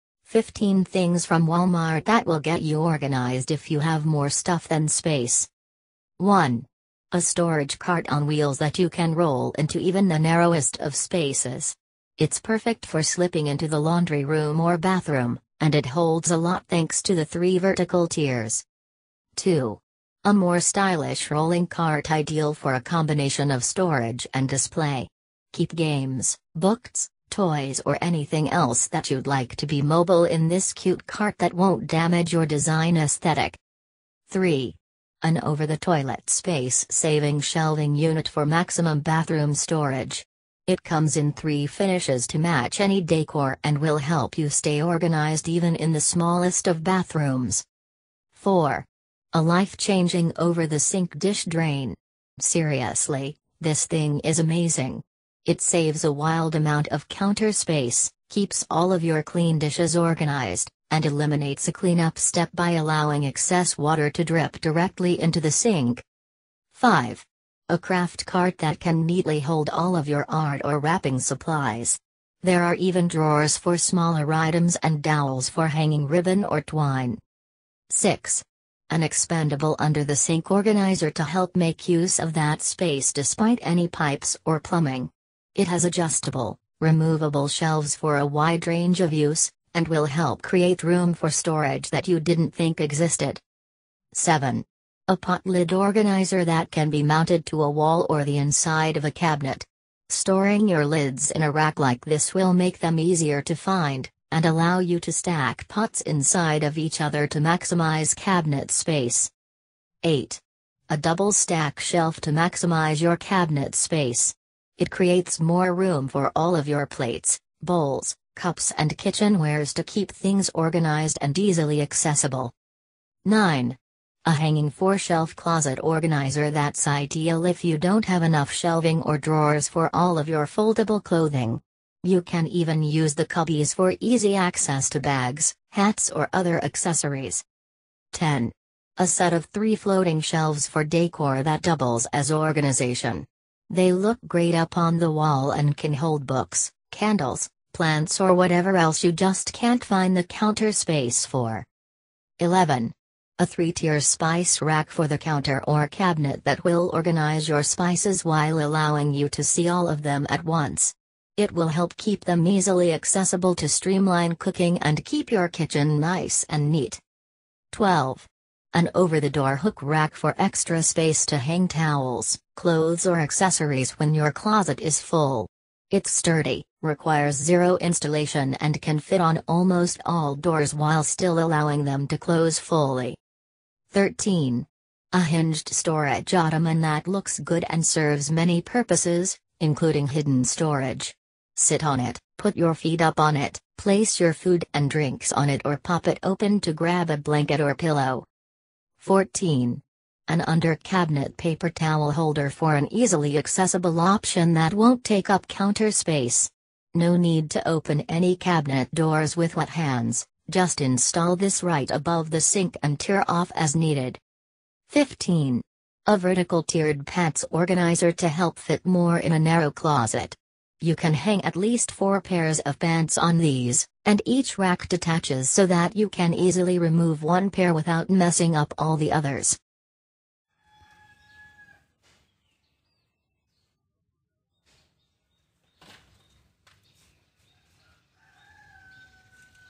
15 Things From Walmart That’ll Get You Organized If You Have More Stuff Than Space
15 Things From Walmart That’ll Get You Organized If You Have More Stuff Than Space

1. A storage cart on wheels that you can roll into even the narrowest of spaces. It's perfect for slipping into the laundry room or bathroom, and it holds a lot thanks to the three vertical tiers.

2. A more stylish rolling cart ideal for a combination of storage and display. Keep games, books, toys or anything else that you'd like to be mobile in this cute cart that won't damage your design aesthetic.

3. An over-the-toilet space-saving shelving unit for maximum bathroom storage. It comes in three finishes to match any decor and will help you stay organized even in the smallest of bathrooms.

4. A life changing over-the-sink dish drain. Seriously, this thing is amazing. It saves a wild amount of counter space, keeps all of your clean dishes organized, and eliminates a clean up step by allowing excess water to drip directly into the sink.

5. A craft cart that can neatly hold all of your art or wrapping supplies. There are even drawers for smaller items and dowels for hanging ribbon or twine.

6. An expandable under-the-sink organizer to help make use of that space despite any pipes or plumbing. It has adjustable, removable shelves for a wide range of use, and will help create room for storage that you didn't think existed.

7. A pot lid organizer that can be mounted to a wall or the inside of a cabinet. Storing your lids in a rack like this will make them easier to find, and allow you to stack pots inside of each other to maximize cabinet space.

8. A double stack shelf to maximize your cabinet space. It creates more room for all of your plates, bowls, cups and kitchenwares to keep things organized and easily accessible.

9. A hanging four-shelf closet organizer that's ideal if you don't have enough shelving or drawers for all of your foldable clothing. You can even use the cubbies for easy access to bags, hats or other accessories.

10. A set of three floating shelves for decor that doubles as organization. They look great up on the wall and can hold books, candles, plants or whatever else you just can't find the counter space for.

11. A three-tier spice rack for the counter or cabinet that'll organize your spices while allowing you to see all of them at once. It'll help keep them easily accessible to streamline cooking and keep your kitchen nice and neat.

12. An over the door hook rack for extra space to hang towels, clothes or accessories when your closet is full. It's sturdy, requires zero installation and can fit on almost all doors while still allowing them to close fully.

13. A hinged storage ottoman that looks good and serves many purposes, including hidden storage. Sit on it, put your feed up on it, place your food and drinks on it or pop it open to grab a blanket or pillow.

14. An under-cabinet paper towel holder for an easily accessible option that won't take up counter space. No need to open any cabinet doors with wet hands, just install this right above the sink and tear off as needed.

15. A vertical tiered pants organizer to help fit more in a narrow closet. You can hang at least four pairs of pants on these, and each rack detaches so that you can easily remove one pair without messing up all the others.